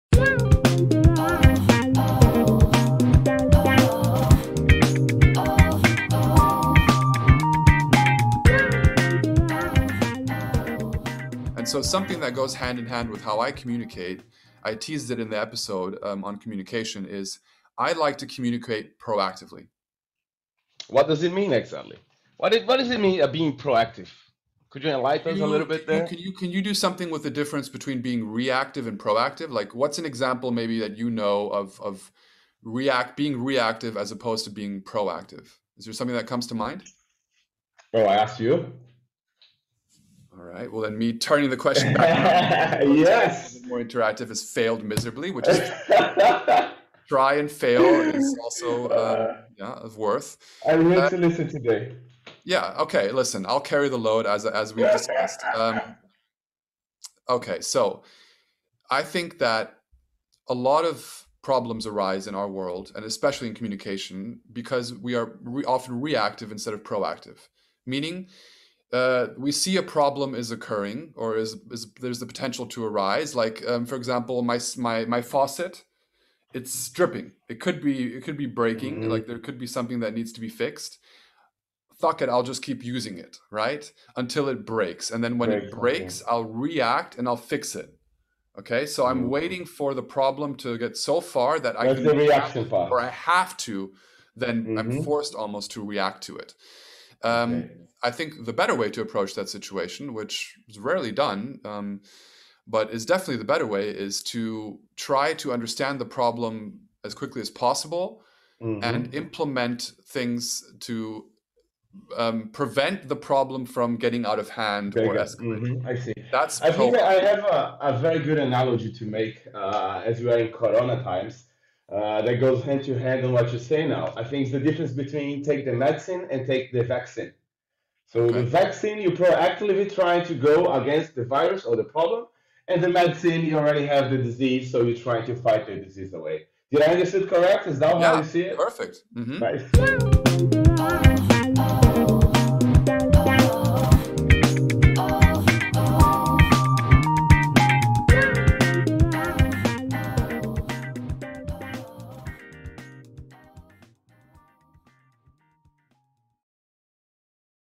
Clip - How To...? - Communication
In the second installment of our new segment "How To...?" we go a bit deeper into the topic of communication! This is a short clip from that episode.